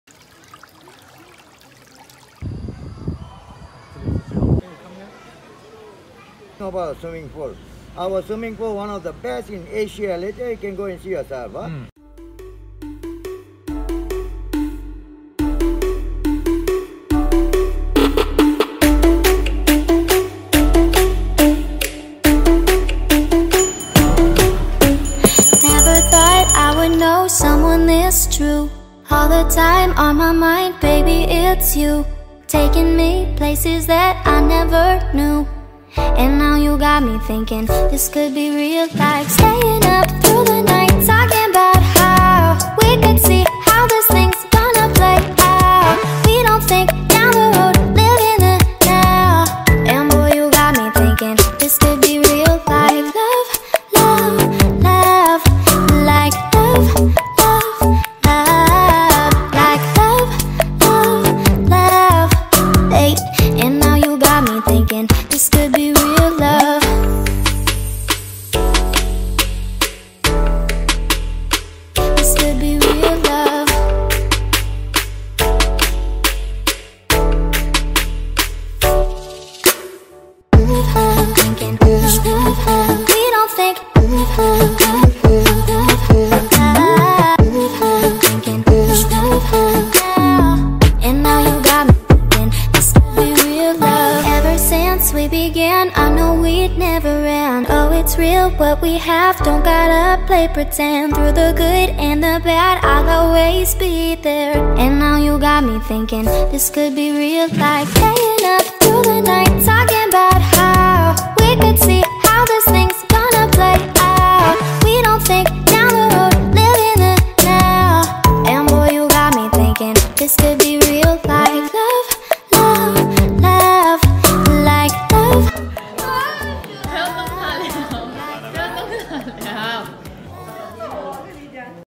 The Haven , Ipoh
Two days chilling trip to take our minds off for awhile.